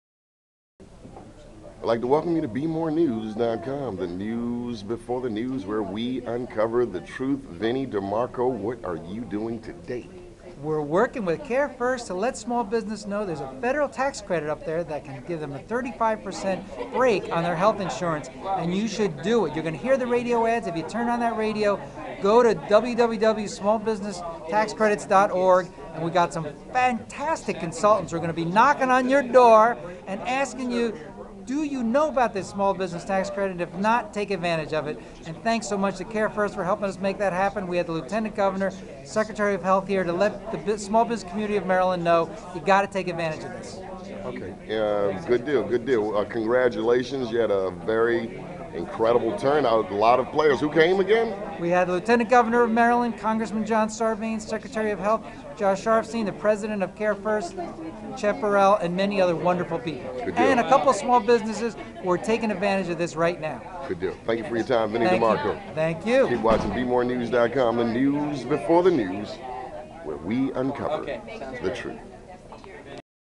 Vinny DeMarco on Health Care for Small Businesses
DeMarco's Health Care for All continues efforts to help all become insured.